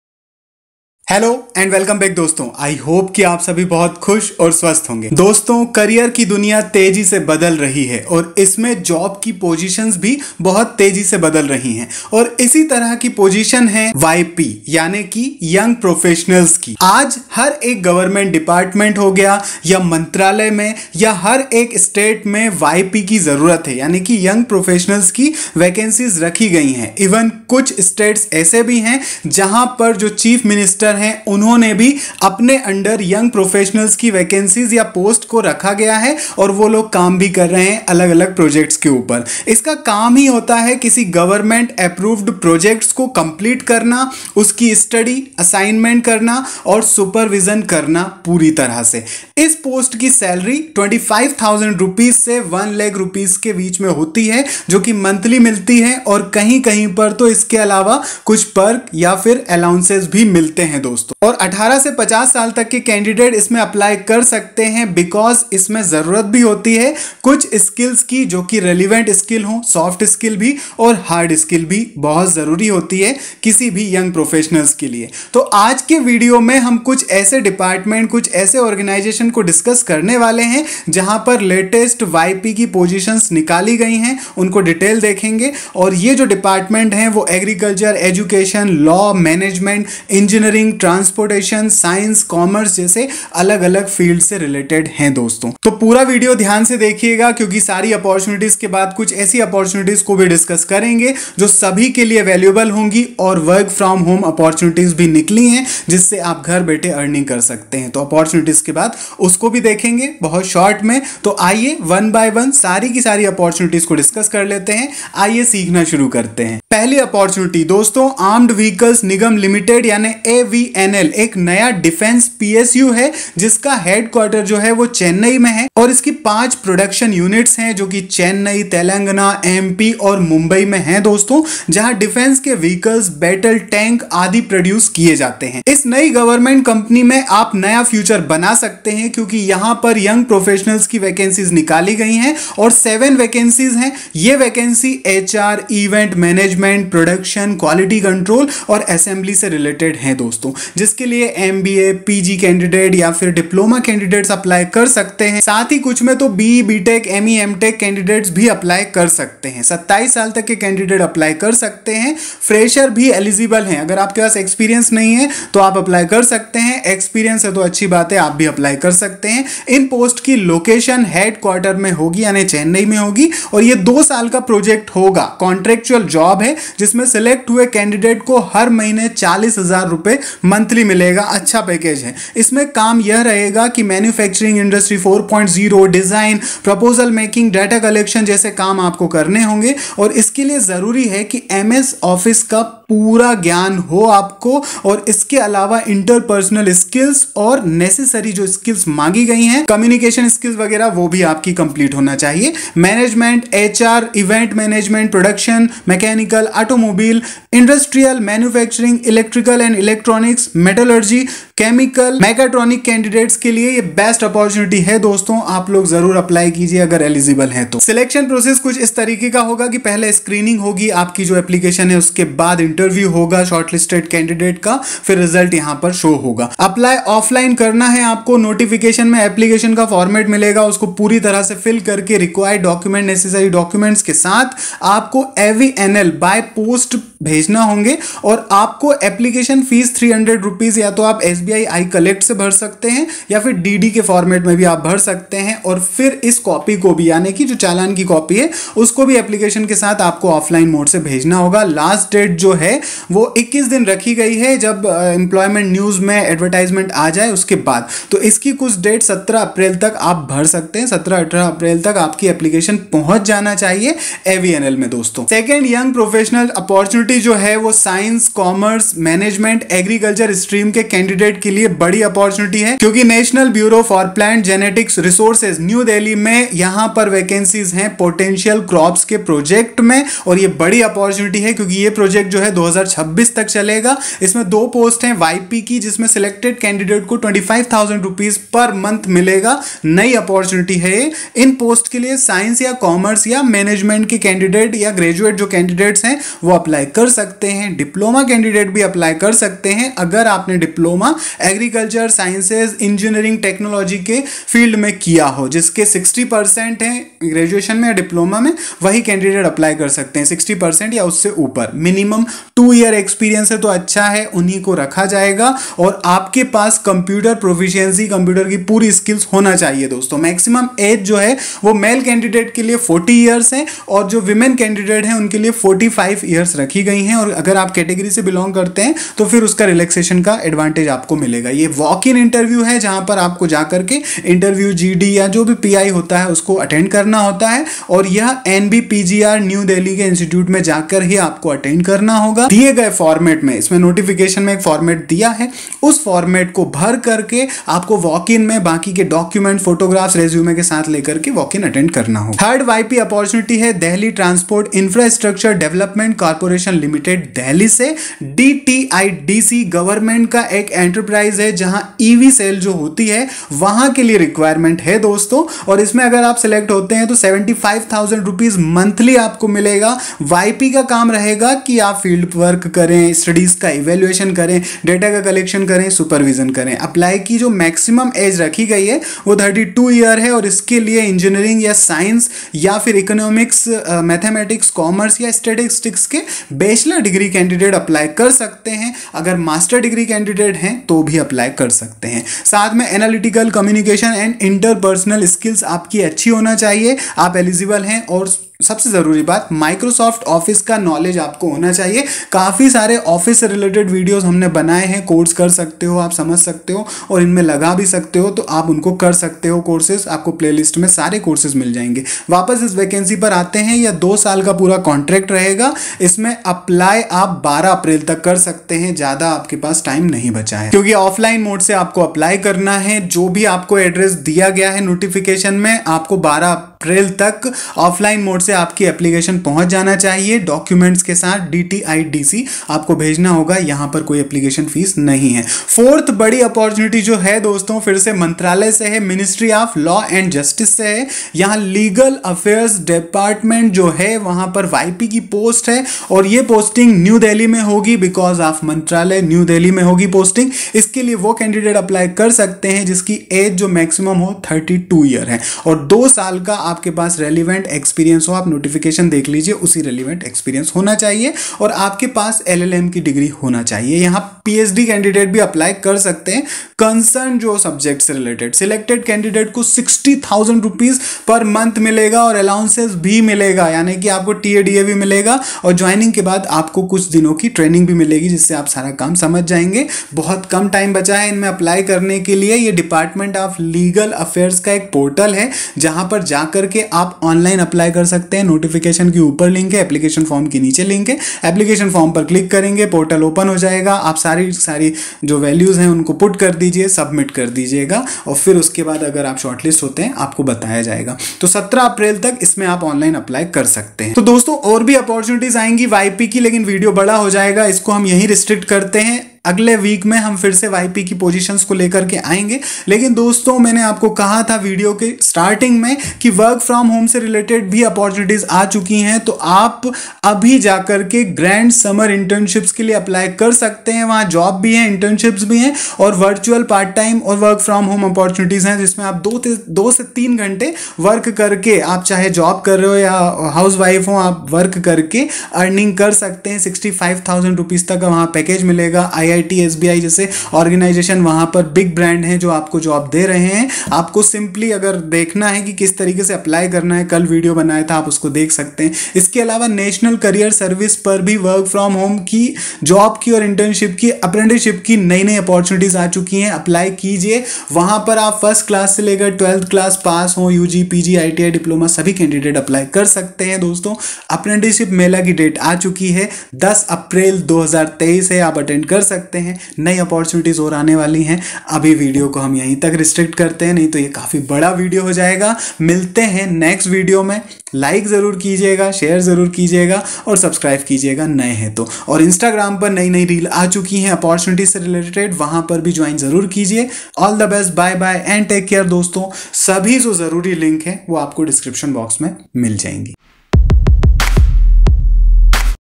Young Professionals Scheme se Paye Rs. 70,000/- Monthly Alert Jobseekers & Students
Today we discuss about YP Scheme, under which jobseekers and students can earn salary, certificates, different perks, by Various government departments and ministries.
-By Dr. Ajay Kumar Choubey